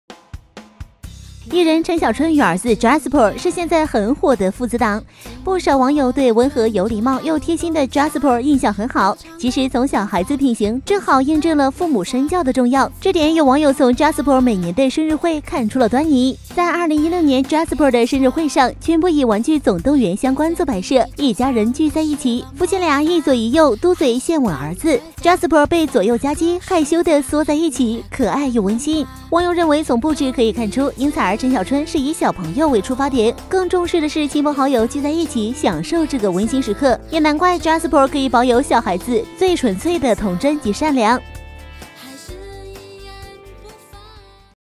【2年前】Jasper三歲生日趴現場曝光 網友：難怪他是小暖男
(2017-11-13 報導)

藝人陳小春與兒子Jasper是現在很火的父子檔，不少網友對溫和、有禮貌又貼心的Jasper印象很好，正好應證了父母身教的重要，這點有網友從Jasper每年的生日會看出了端倪。